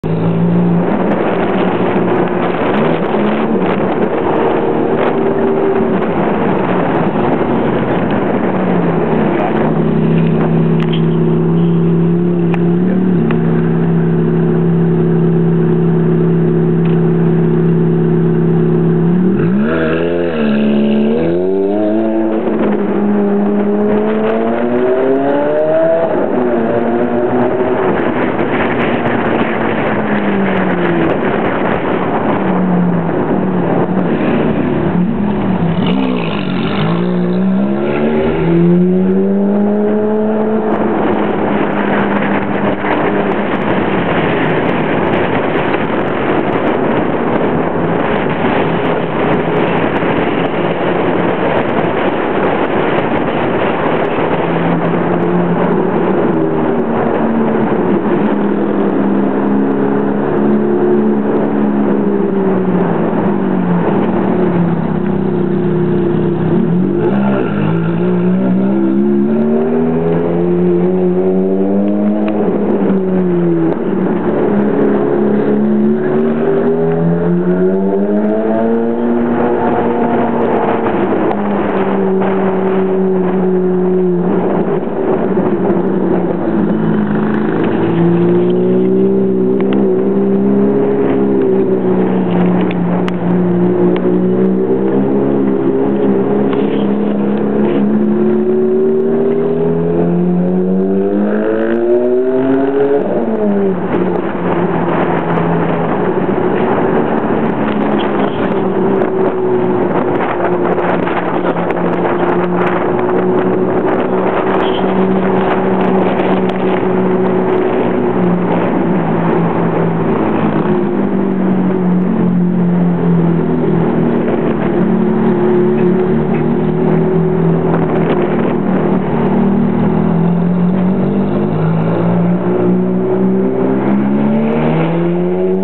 Ferrari F430 Novitec Chased around Monaco - Amazing Sound
A video of me following a lovely F430 around Monaco in my 300ZX Twin Turbo in June 2007.  We traveled down from Pensby in the Wirral to stay with my family in the south of France.